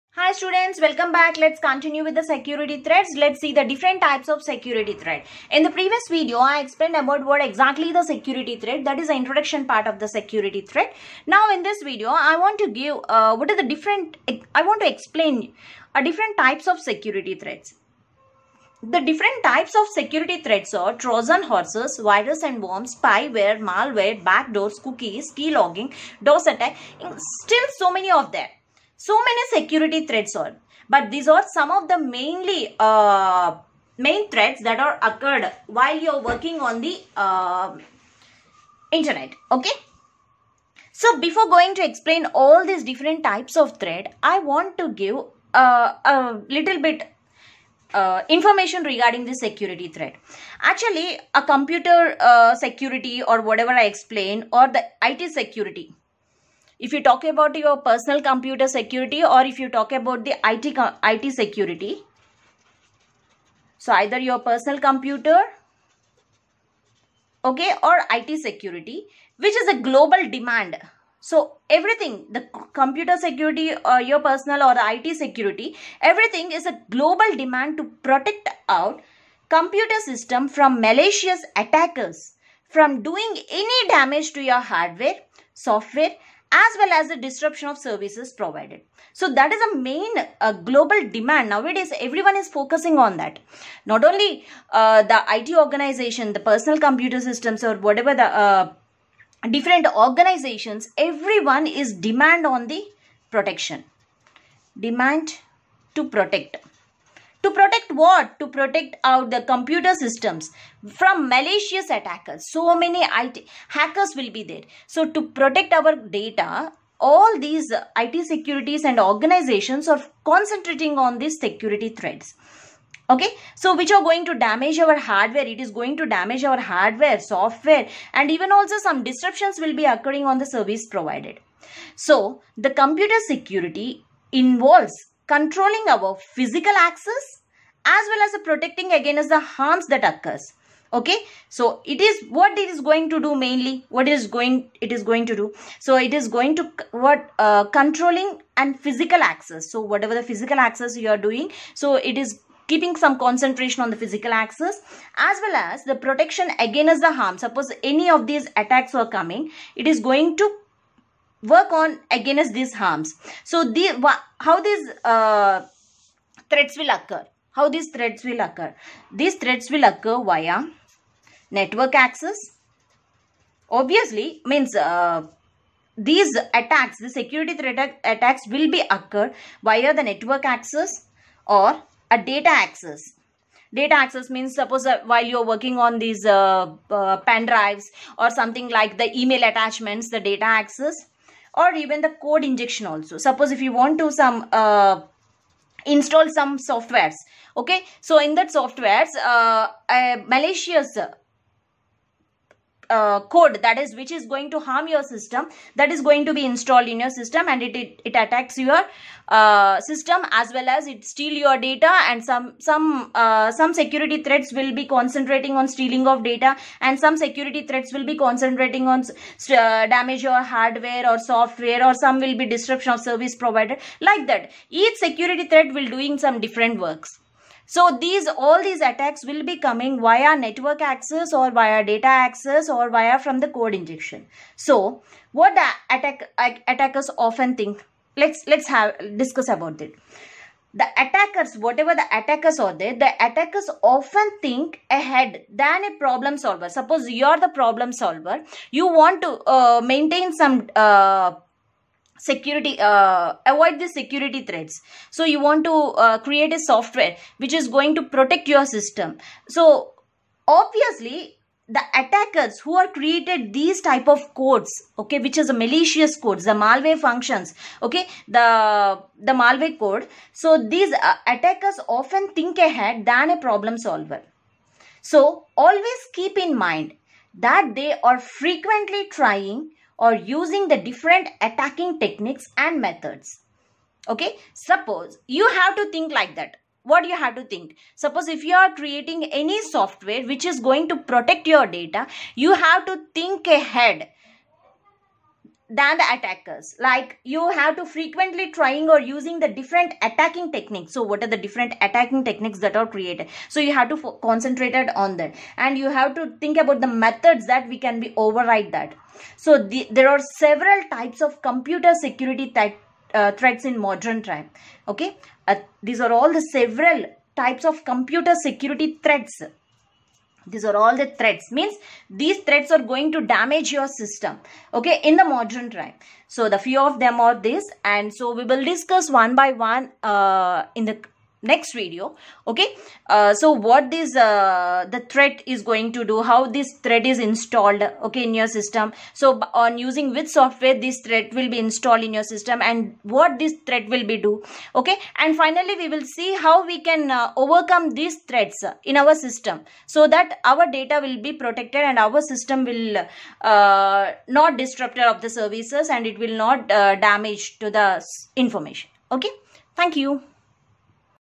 Types of Security Threats | Part-1/3 | WT | Web Technology | Lec - 52 | Bhanu Priya
Web Technology ( WT )  - Security Threats
Overview of all Types of threats